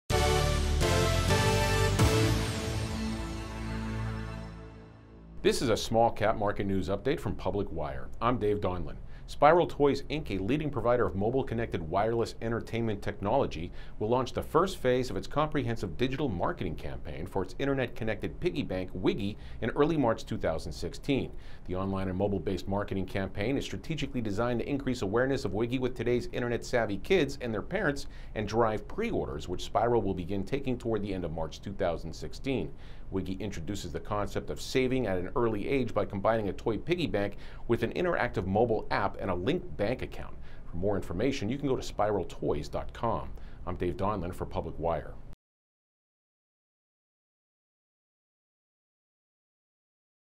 Spiral Toys, Inc
Spiral Spiral Toys, Inc. (OTCQB: STOY) is a California-based company with a business, founded by a former senior executive of Disney and Sony, focused on developing and marketing products and mobile applications in the mobile-connected space. Spiral’s mobile-connected entertainment platform connects physical items to today’s top mobile devices through wireless technologies, creating a new kind of interactive user experience

More stock markets today and investor relations information along with stock news today on Spiral Spiral Toys, Inc. can be viewed